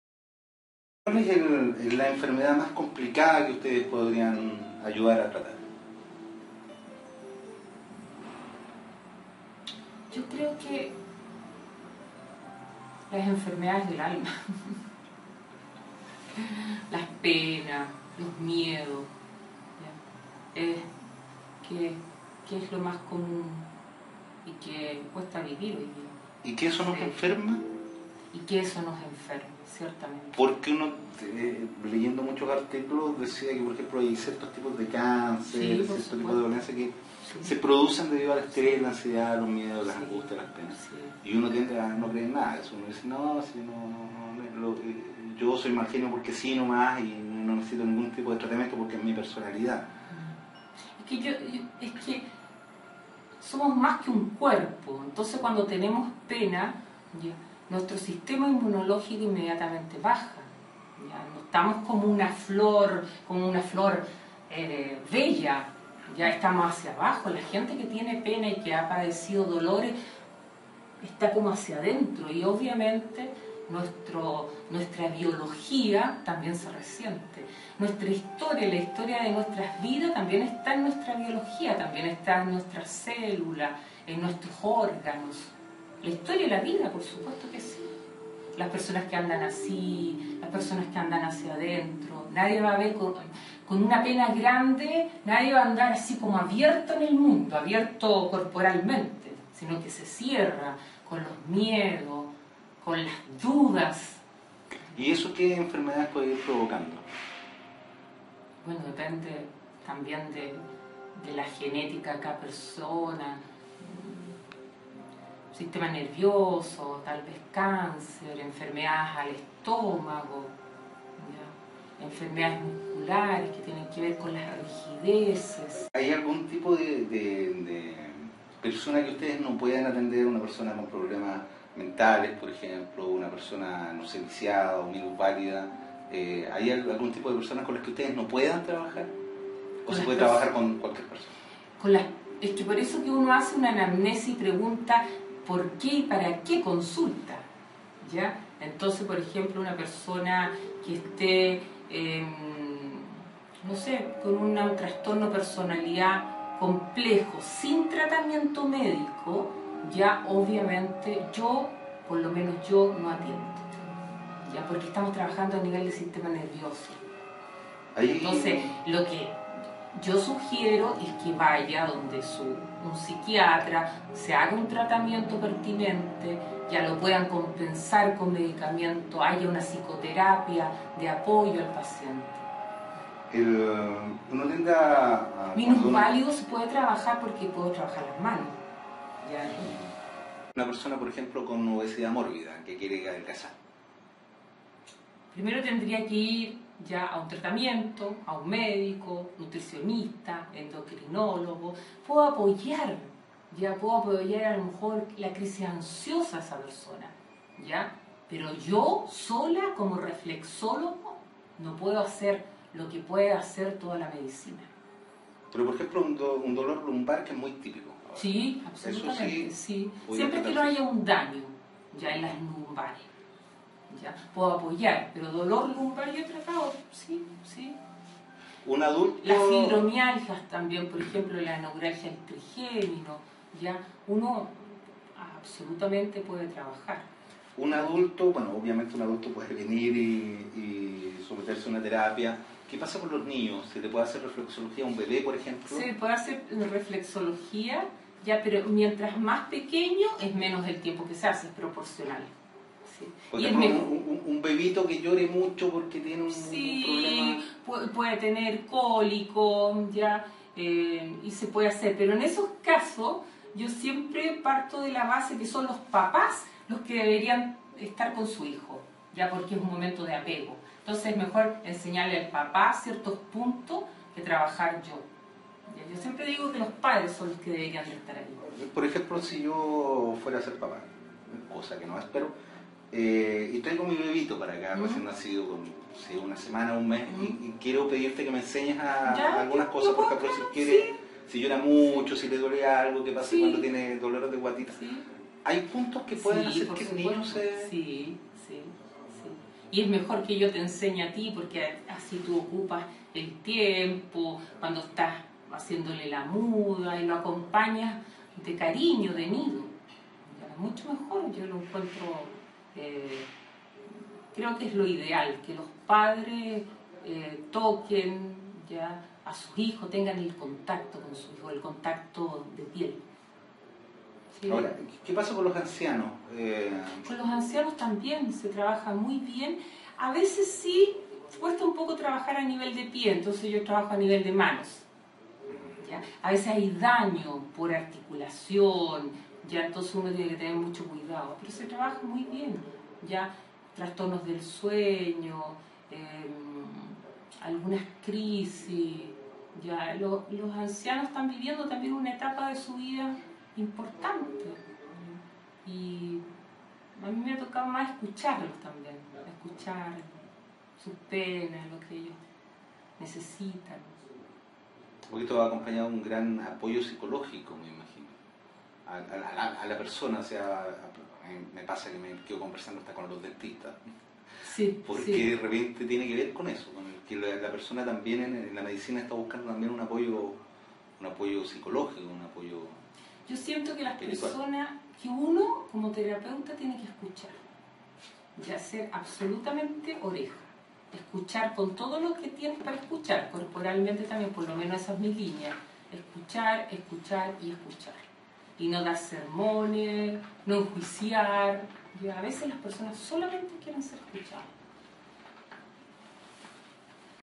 Claudia Sepúlveda. Reflexología. Tercera parte.mp4
Claudia Sepúlveda es reflexóloga, con muchos años de experiencia y sólida formación académica, nos cuenta de que se trata esta disciplina.